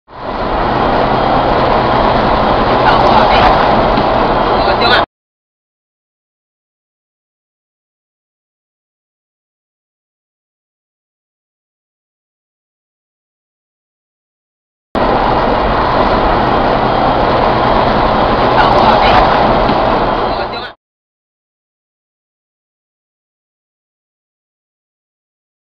貨車逼車肇逃 司機逾期到案判免罰 警怨：尊重（國道七隊提供）
開惡例？1輛大貨車在國道3號彰化路段疑因惡意逼車，造成小貨車翻覆駕駛受傷，法院認為警方舉發逾3個月，日前判大貨車司機免罰，國道七隊警方今揭示當時影片強調，事故發生後大貨車肇事逃逸，過濾比對1個月後查出司機行蹤，司機卻消極拖延，遲至3個多月後才到案，明顯有故意脫罪之嫌，對於法院判決，警方只能予以：「尊重！」